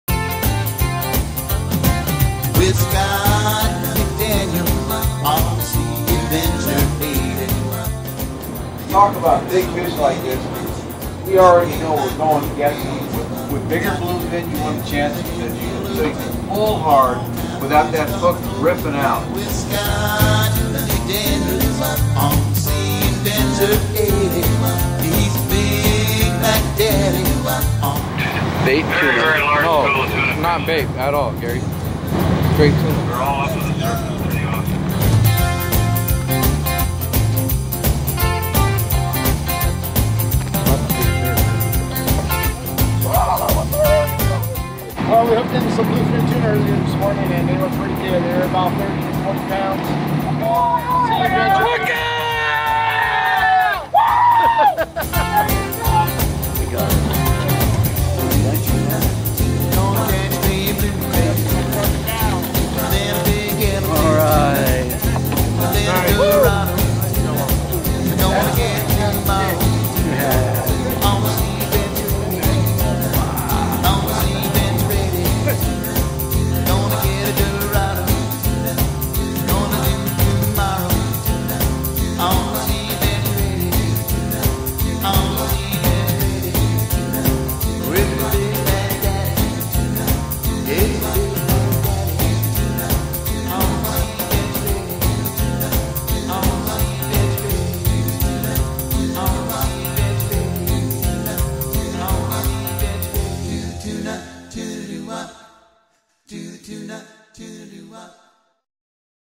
Sea Adventure 80 "Big fish bite early this Season on a 1.5 day trip"
Some highlights of a 1.5 day trip June  20th 2015 with captain Scott McDaniels, and his outstanding crew,  aboard the real Sea Adventure 80. Book early if you want to fish with the best! Trips selling fast.